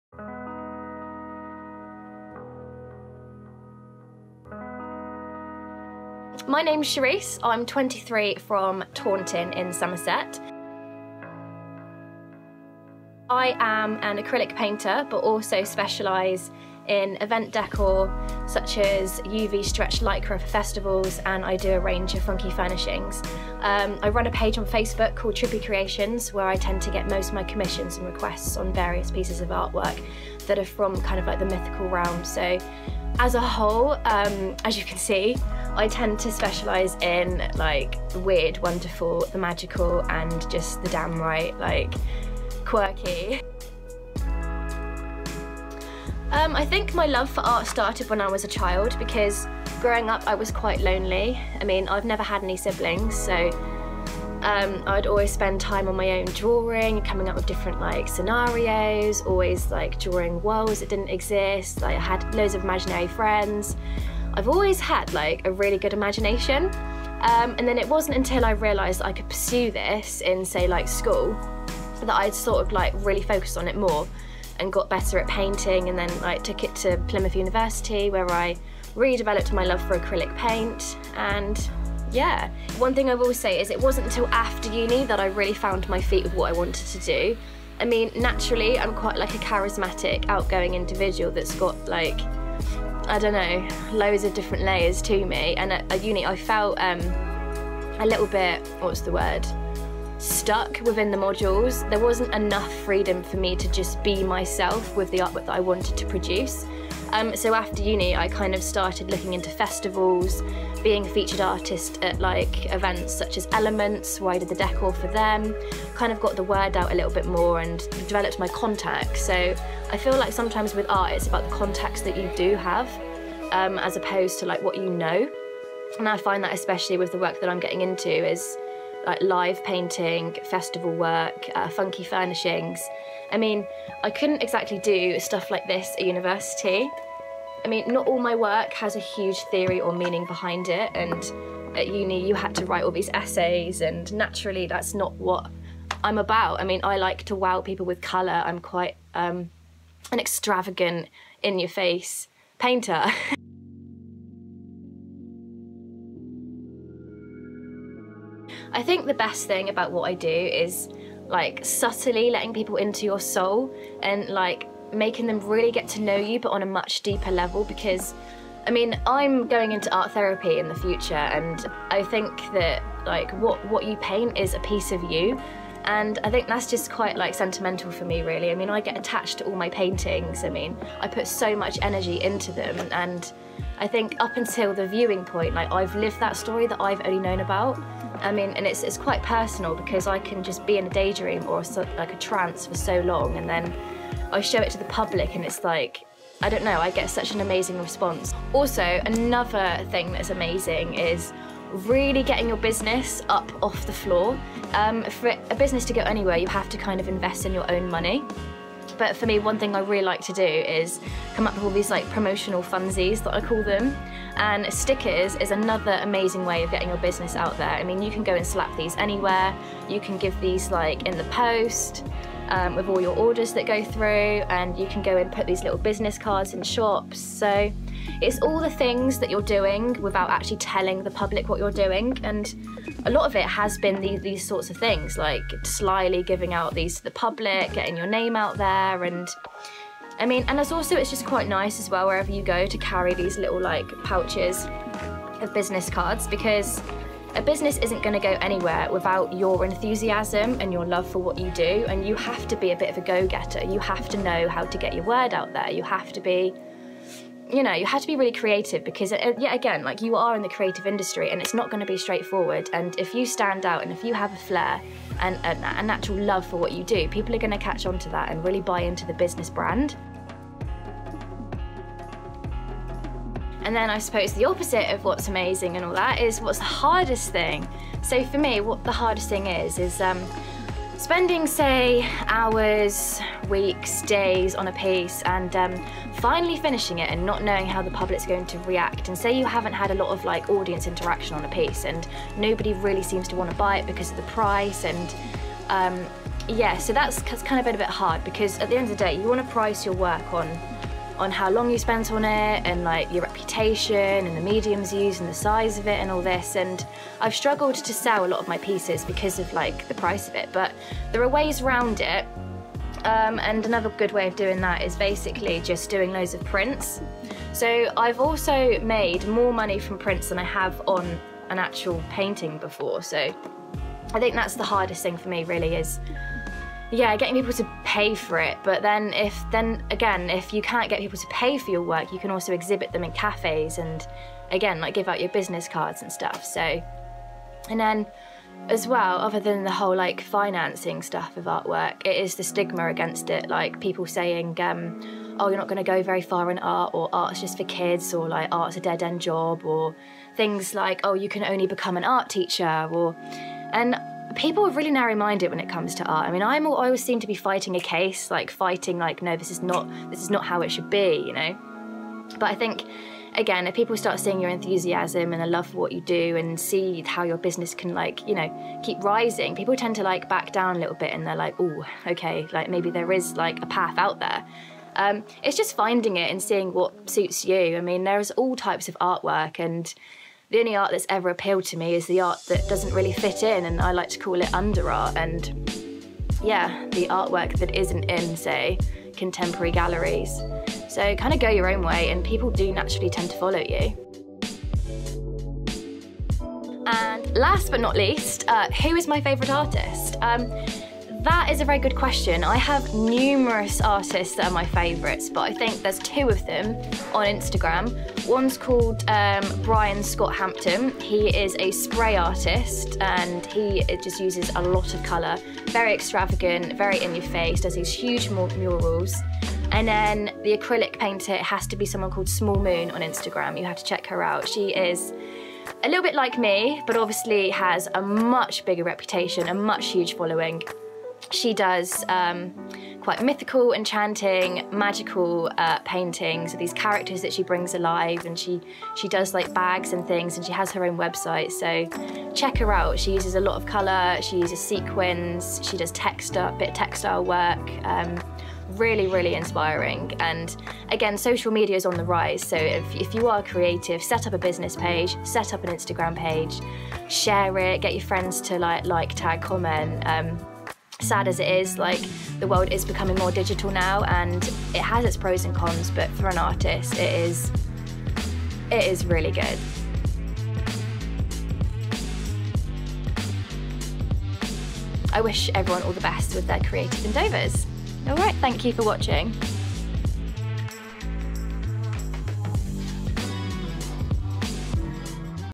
Our Artists, Episode 4: Cherise Woods
Cherise talks about her life as a young, upcoming artist.

There's something for everyone in this, whether you're looking to find out more about her work, life as an artist more generally or even advice on how to promote yourself in any small, creative business.